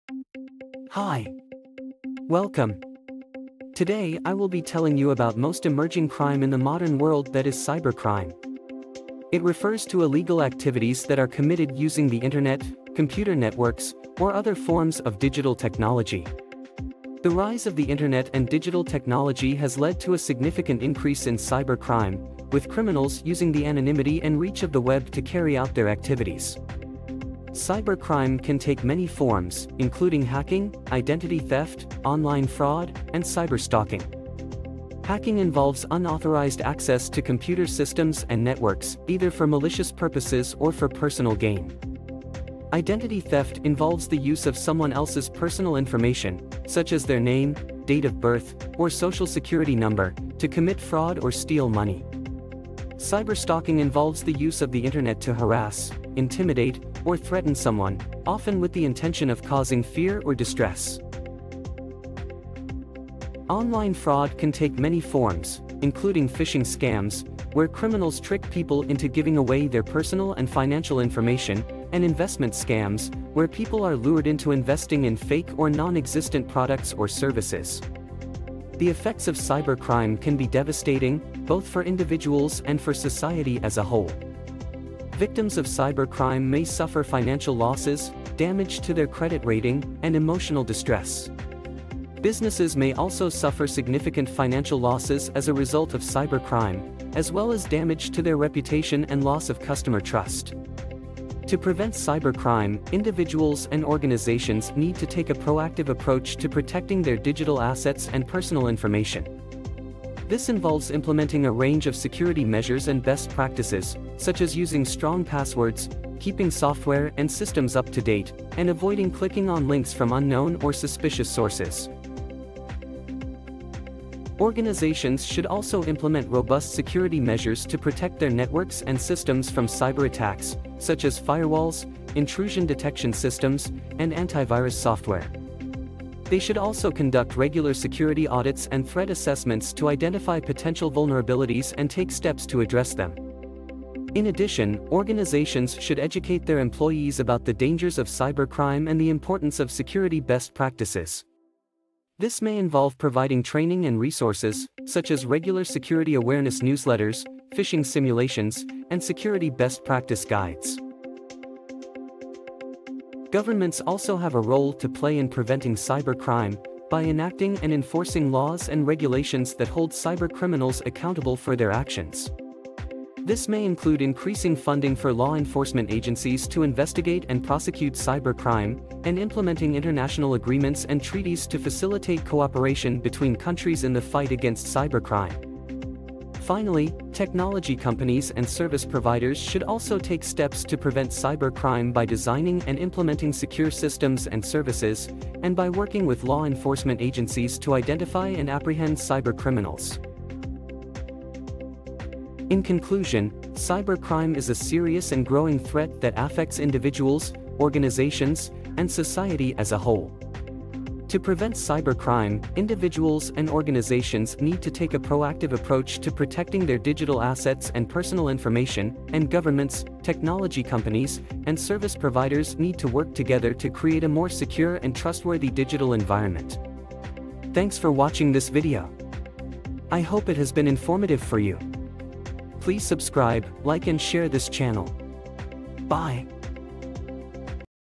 Cybercrimes and prevention
Introduction to Cybercrime, its types and prevention.